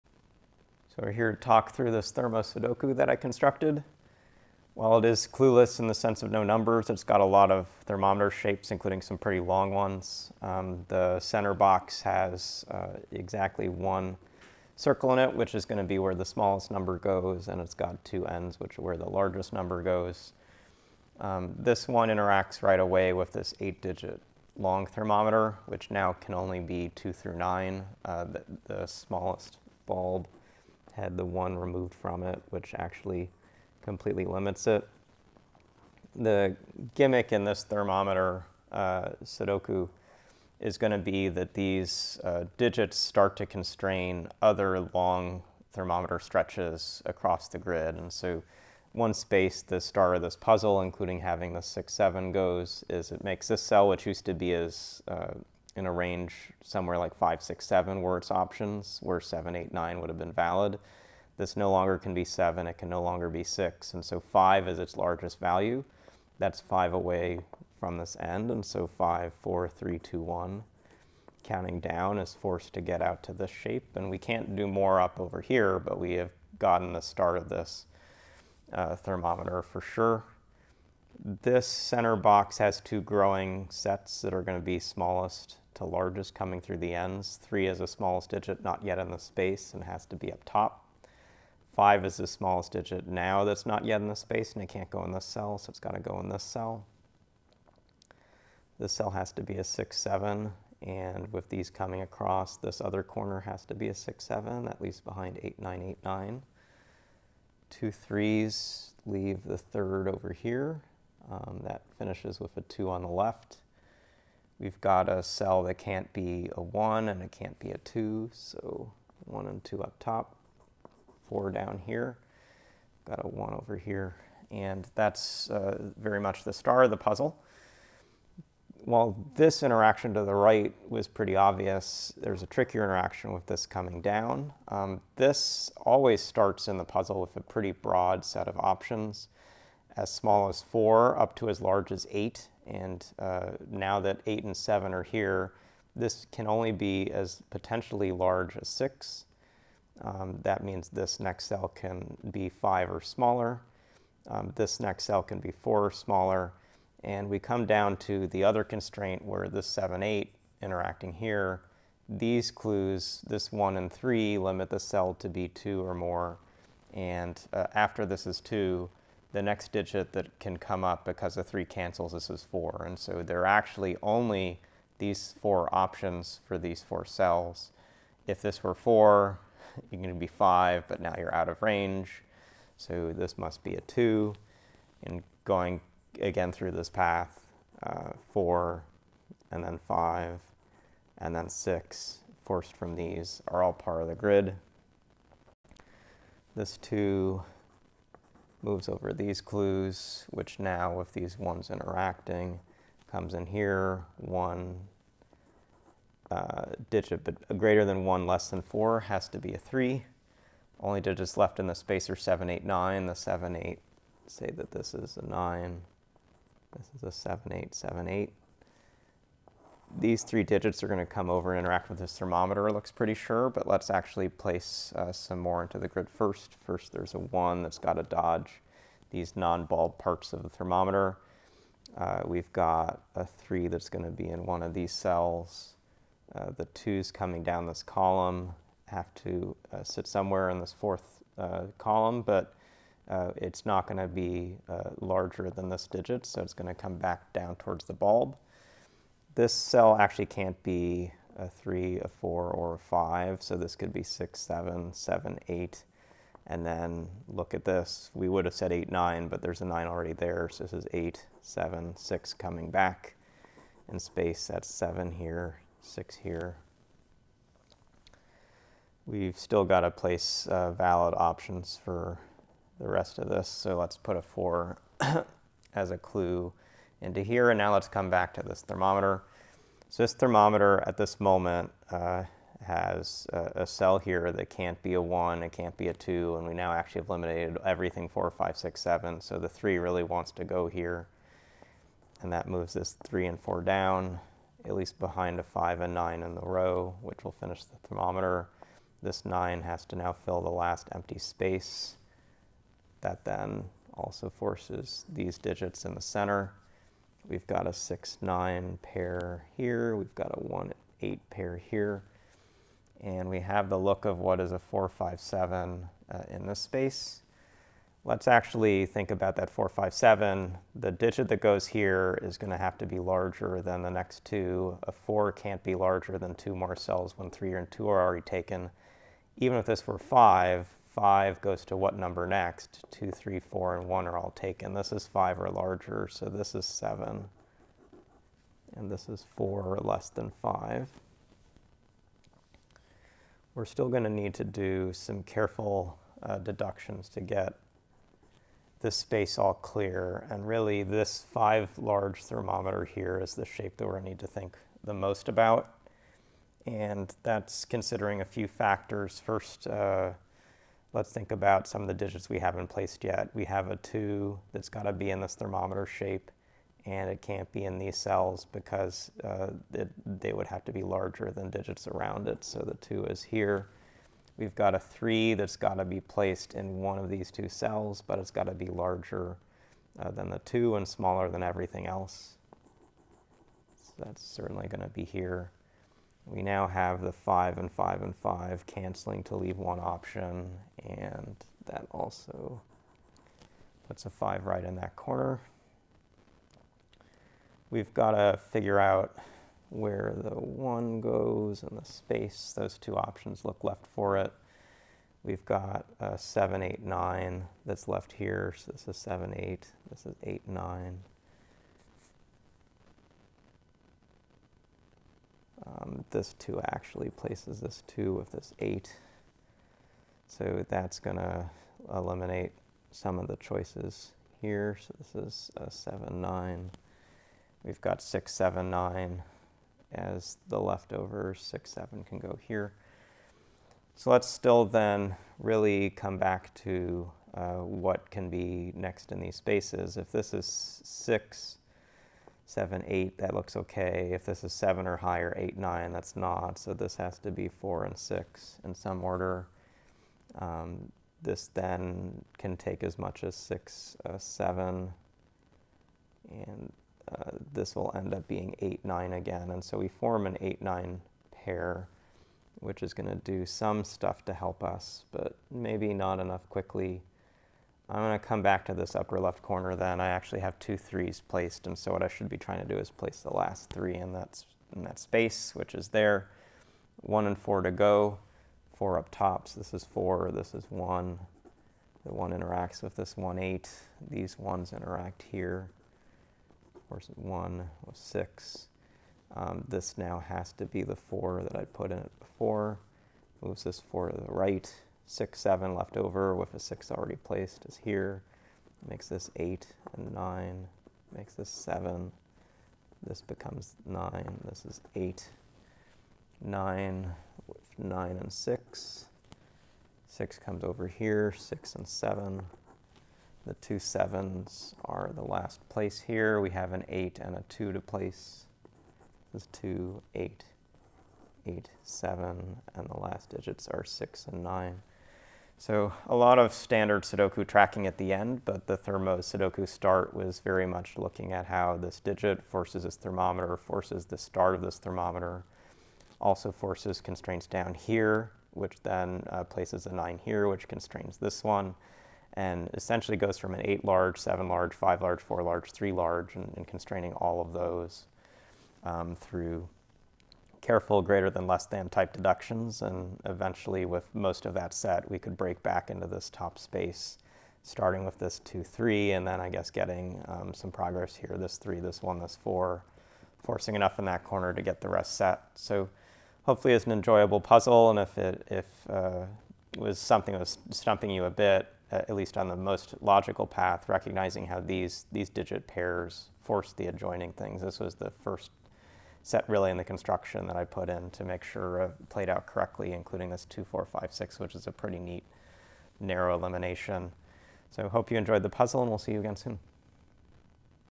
GMPuzzles - 2020/11/28 - Thermo-Sudoku by Thomas Snyder
Could you find the connections between several of the thermometers to solve this tough Thermo-Sudoku by Thomas Snyder? This walkthrough will show you the way.